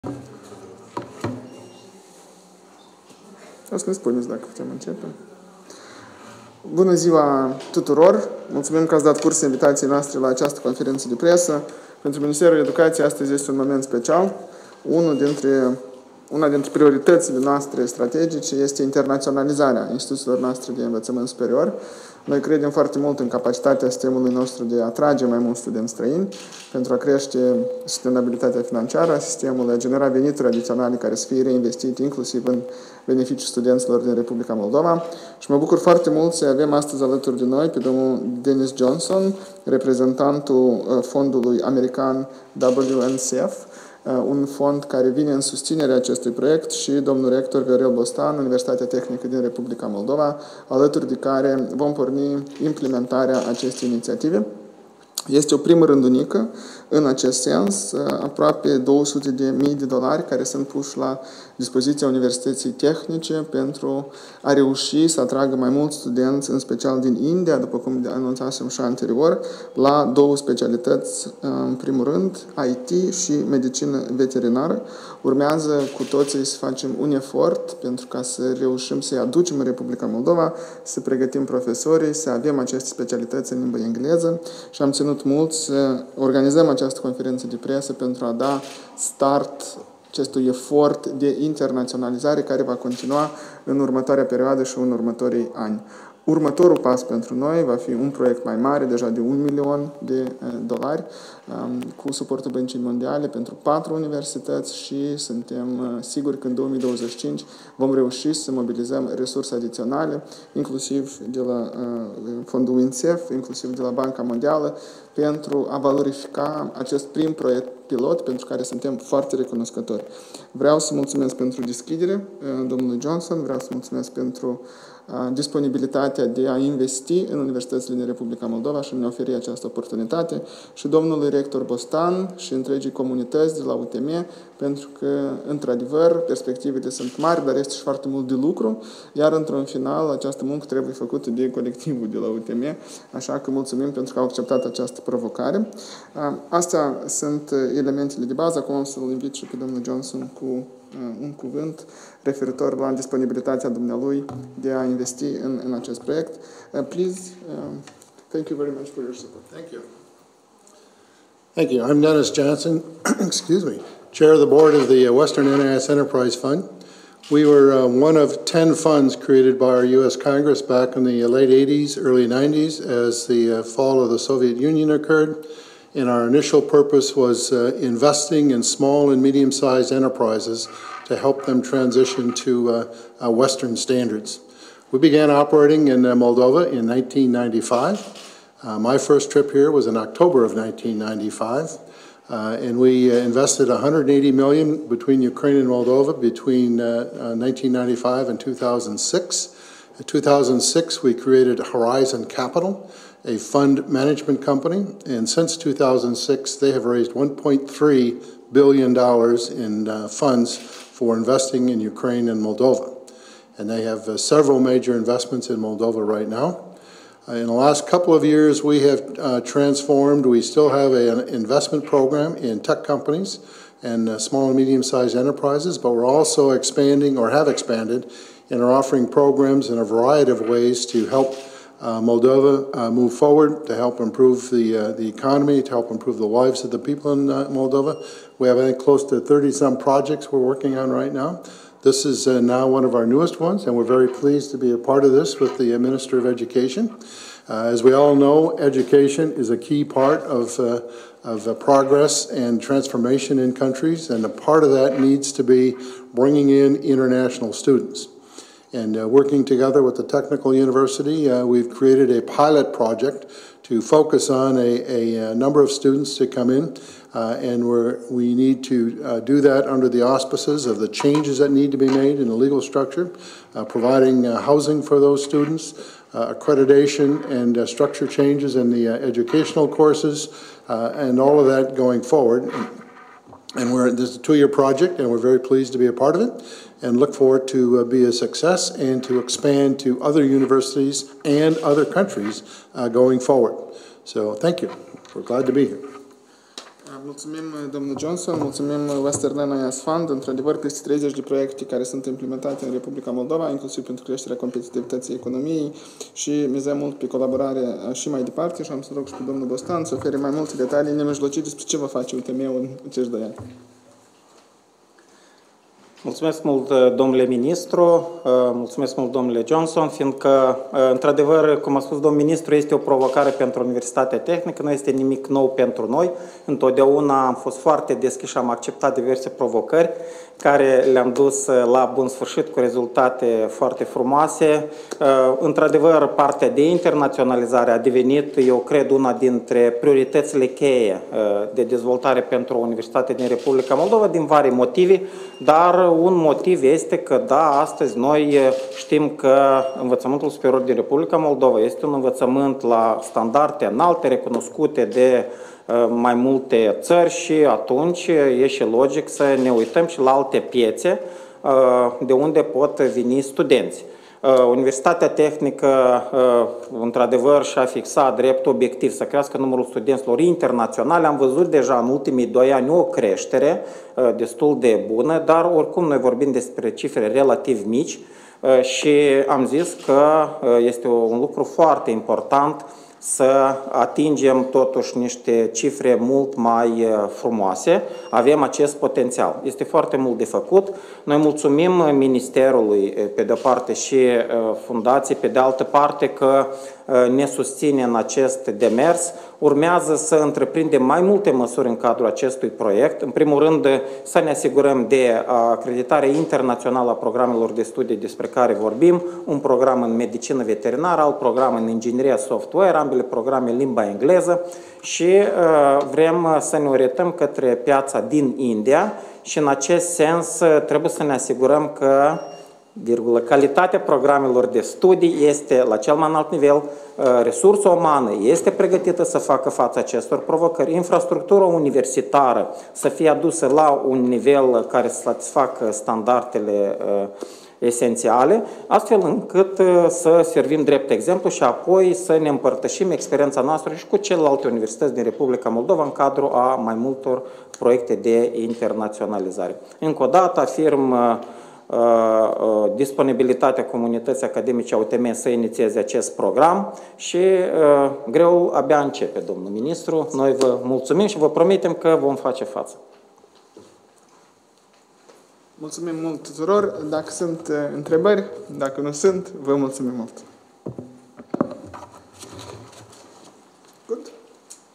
Semnarea Memorandumului privind internaționalizarea învățământului superior din Republica Moldova
Semnarea Memorandumului de Înțelegere între Ministerul Educației și Cercetării, Fondul Western NIS Enterprise, Universitatea Tehnică din Moldova și Fundația pentru Dezvoltare din Republica Moldova, privind internaționalizarea învățământului superior din Republica Moldova